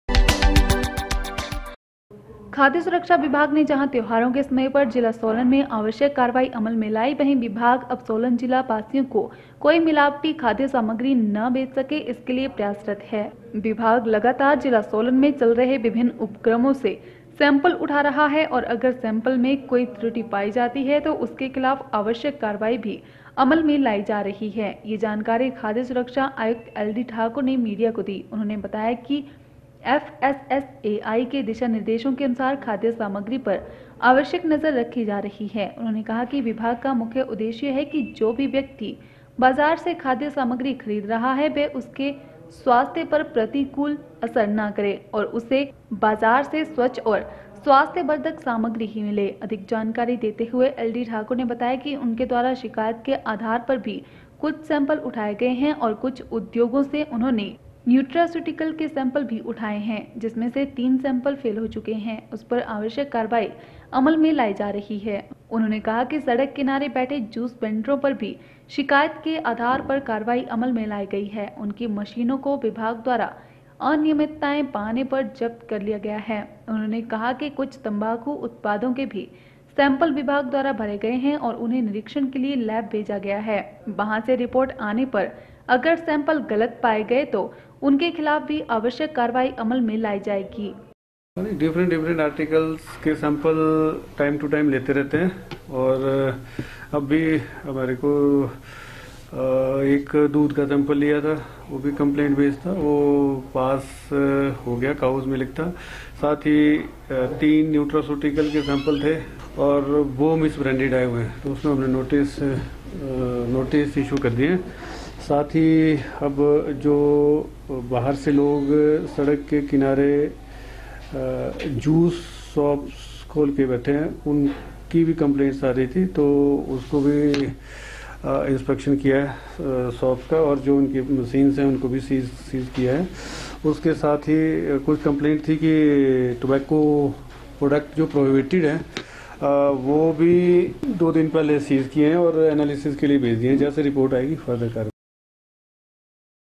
5 dec 8 जिला सोलन में तीन न्यूट्रास्यूटिकल सैम्पल हुए फेल : एलडी ठाकुर
खाद्य सुरक्षा विभाग ने जहाँ त्योहारों के समय पर जिला सोलन में आवश्यक कार्रवाई अमल में लाई |  वहीँ विभाग अब  सोलन जिला वासियों को कोई  मिलावटी खाद्य सामग्री ने बेच सके इस के लिए  प्रयास रत है | विभाग लगातार जिला सोलन में चल रहे विभिन्न उपक्रमों से सैम्पल उठा रहा है  और अगर सैम्पल में कोई त्रुटि पाई जाती है तो उसके खिलाफ आवश्यक कार्रवाई भी अमल में लाई जा रही है | यह जानकारी खाद्य सुरक्षा आयुक्त एलडी ठाकुर ने मीडिया को दी